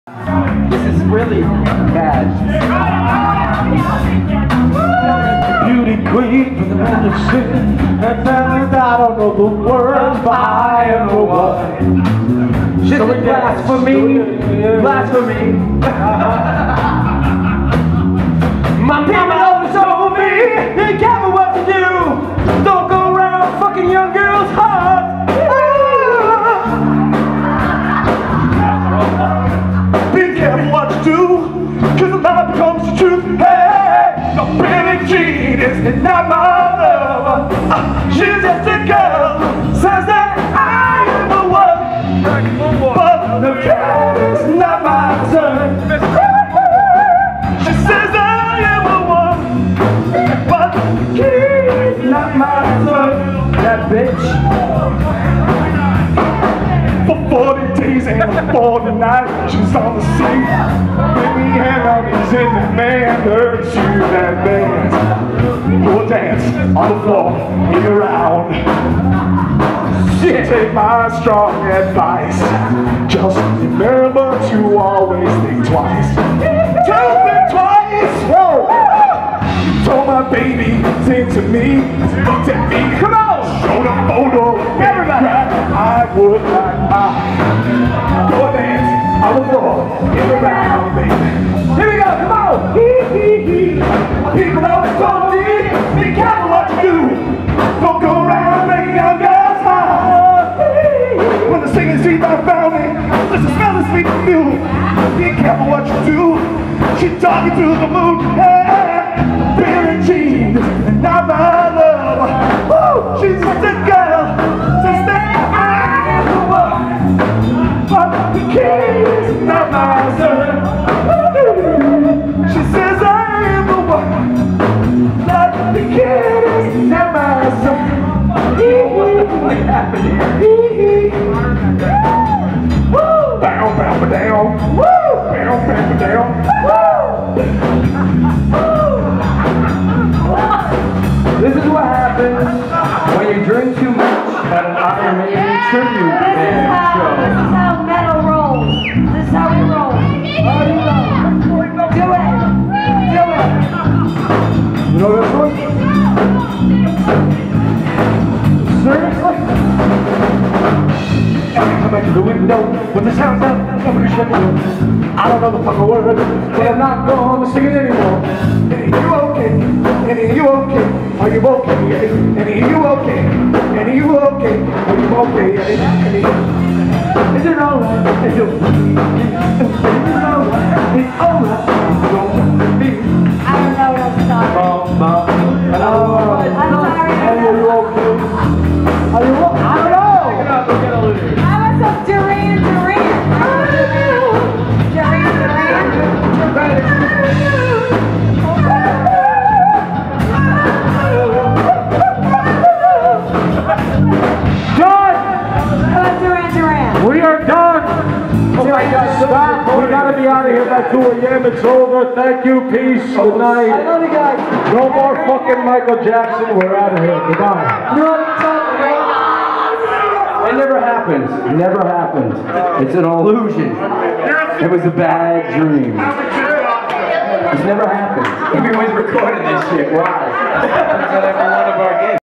Wrathchild Iron Maiden Tribute(w/ Marc Lopes) - "Michael Jackson Medley" LOL
Aftershow drunken fun at Cook's Cafe in Naugatuck, CT on August 17, 2013